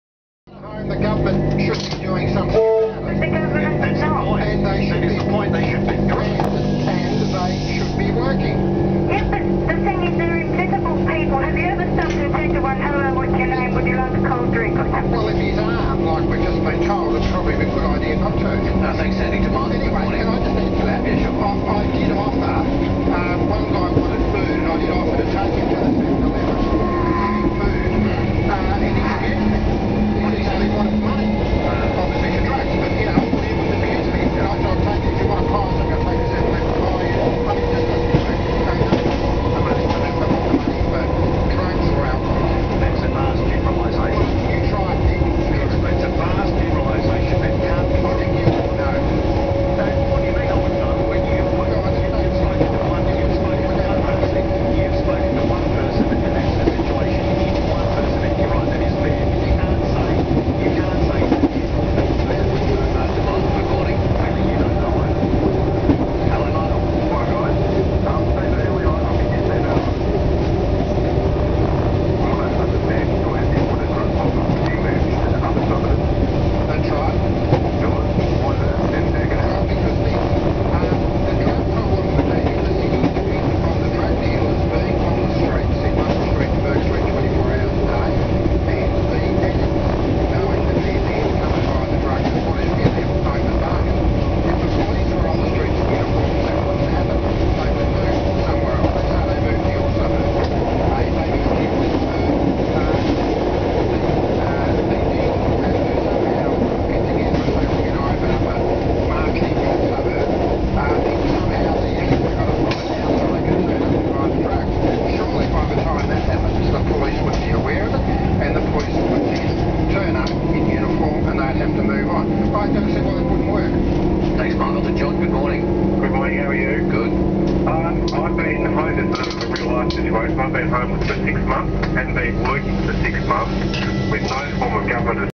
Holmesglen To Jordanville On A 3 Car Comeng...
Sunday morning run from Holmesglen To Jordanville on a 3 car Comeng.